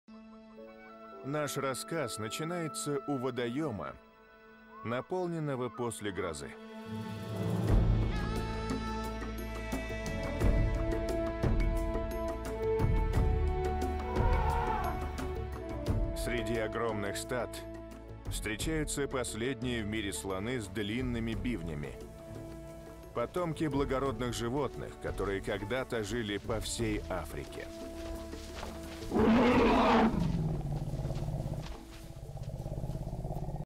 SEH: Стада диких африканских слонов в саванах
Африканские слоны с большими бивнями в большом стаде